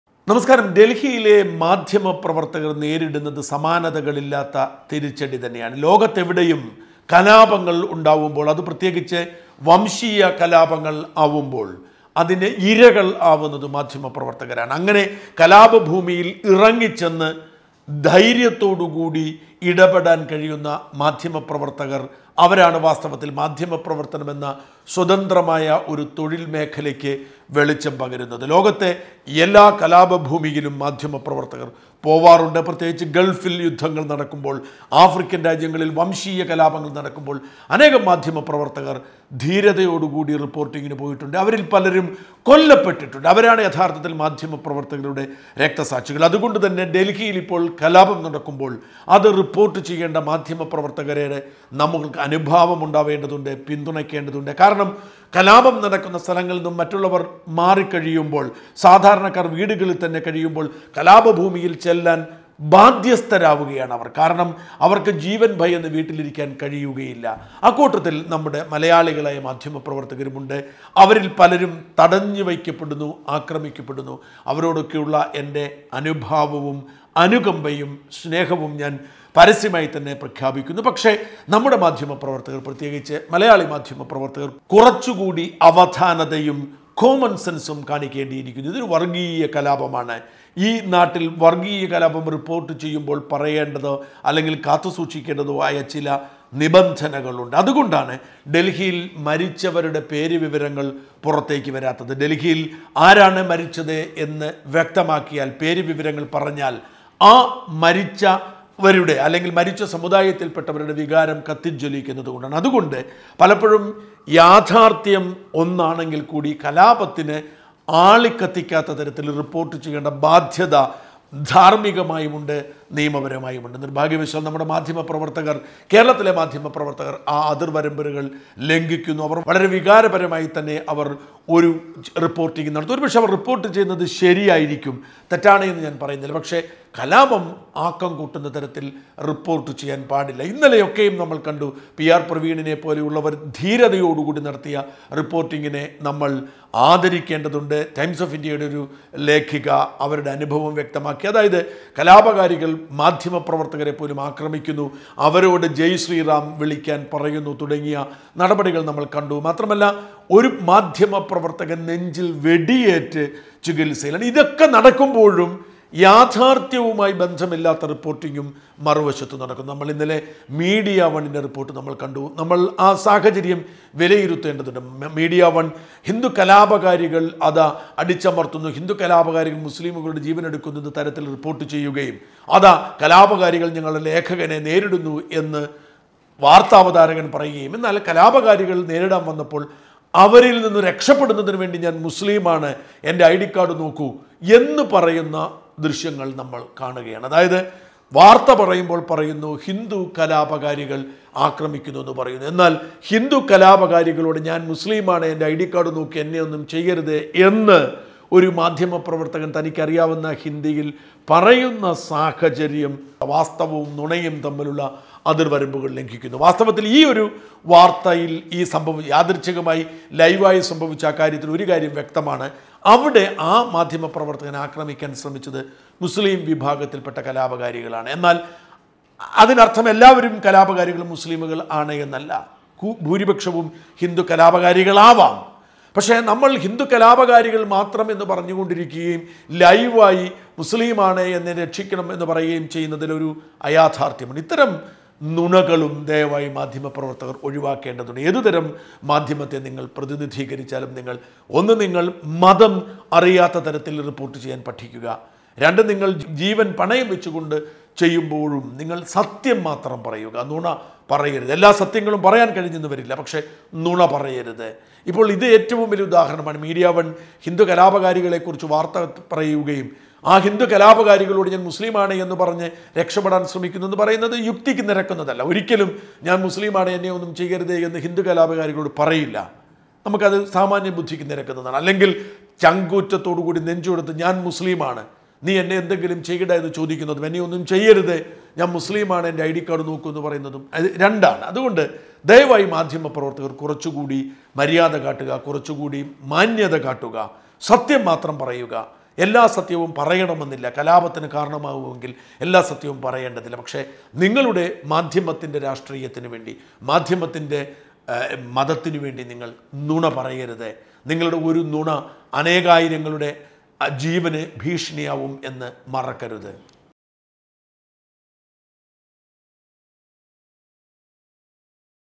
ഡല്‍ഹിയിലെ പത്രക്കാരും കരുതല്‍ എടുക്കട്ടെ.. I News reporting
ഞാന്‍ മുസ്ലീമാണ് എന്ന് പറഞ്ഞ് ഹിന്ദു കലാപകാരികളെ നേരിടുമ്പോള്‍...
ഡല്‍ഹിയിലെ പത്രക്കാരും കരുതല്‍ എടുക്കട്ടെ..